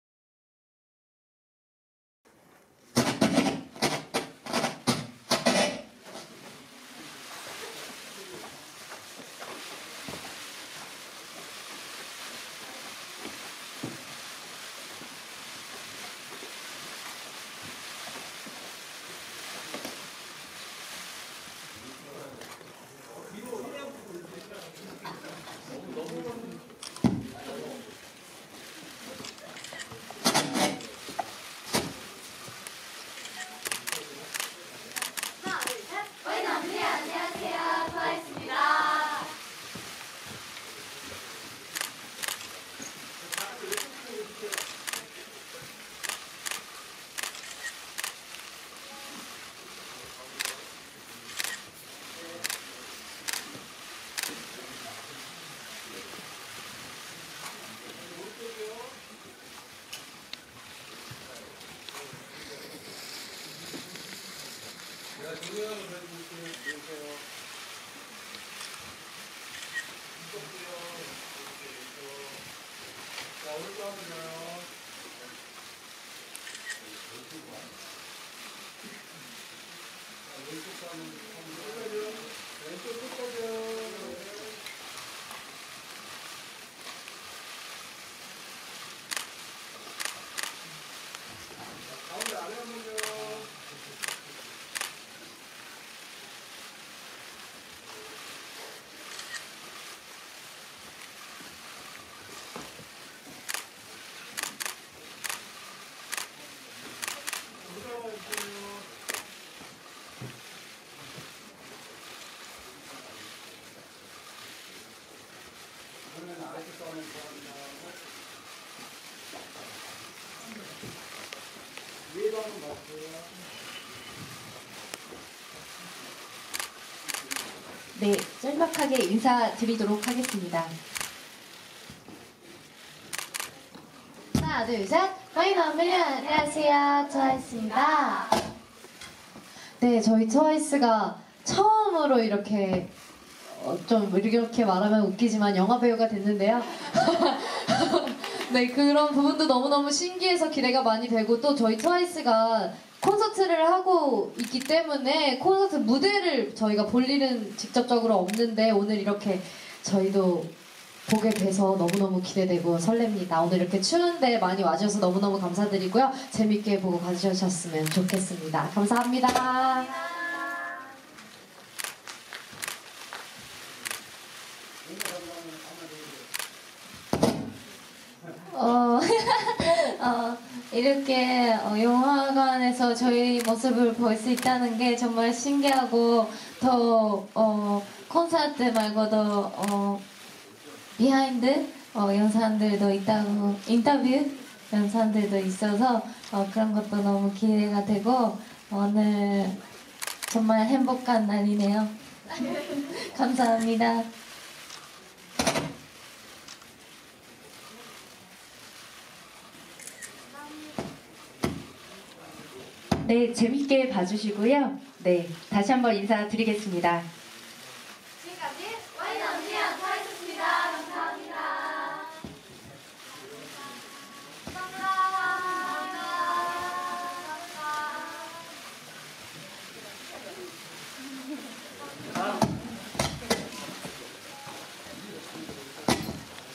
181206 영화 '트와이스랜드' 시사회에 참석한 트와이스(TWICE) 무대인사 직캠 영상
6일 오후 서울시 용산구 CGV용산아이파크몰에서 영화 '트와이스랜드(TWICELAND)'의 언론시사회가 열렸다. 시사회 전 트와이스가 무대인사 직캠 영상